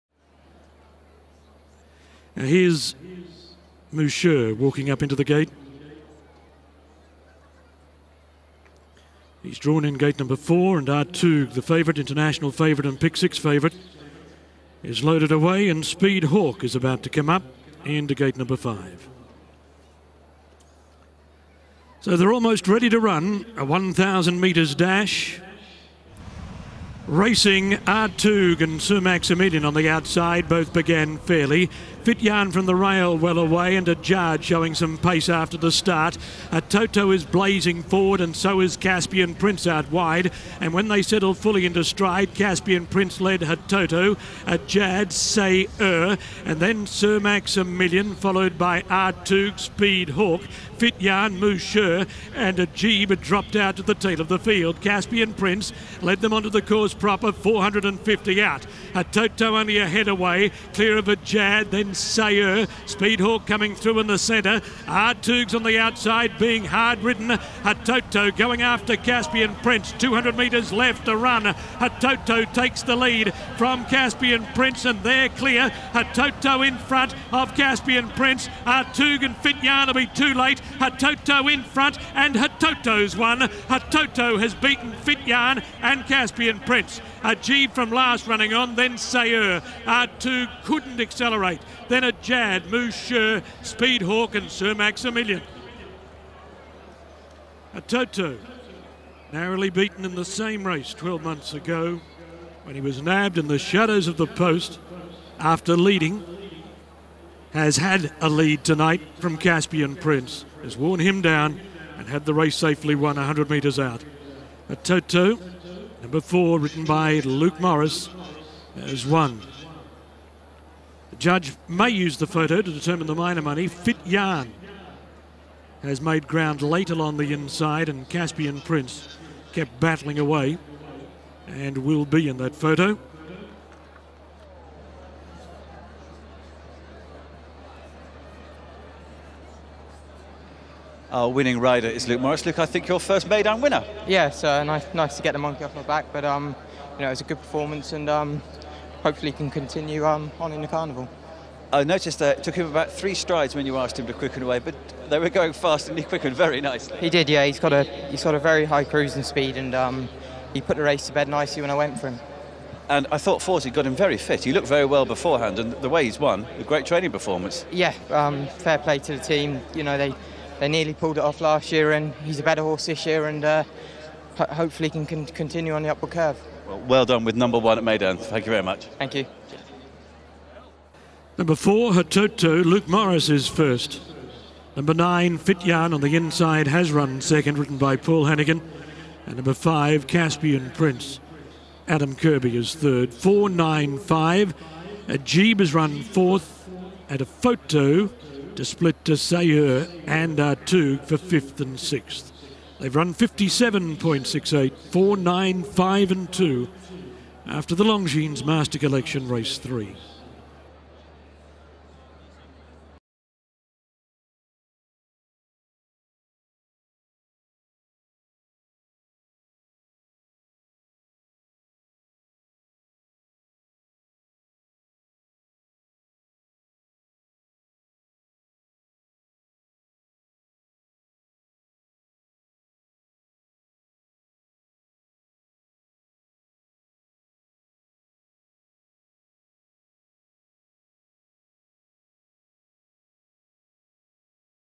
08.01.2015 Meydan (Dubai/UAE) 3.Race Longines Master Collection - Handicap 1.000 m
(4yo+)
1. Hototo (4)
2. Fityaan (9)
3. Caspian Prince (5)
4. Ajeeb (2)